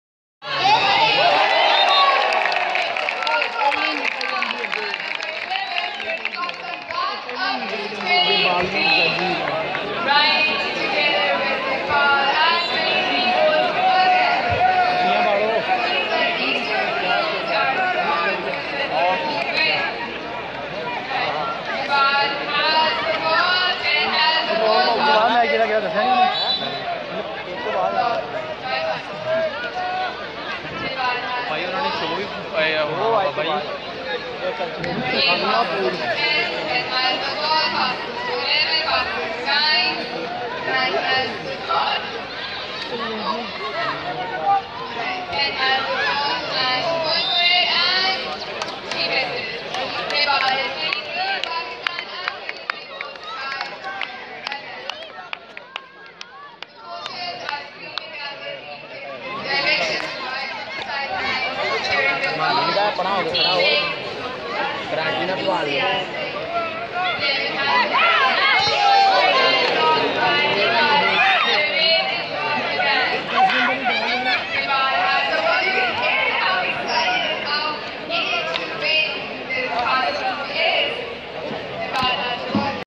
Pakistan-vs-India Women International Hand Ball Match at Faisalabad
International women hand ball championship 2016 is held on university of agricultural Faisalabad Pakistan...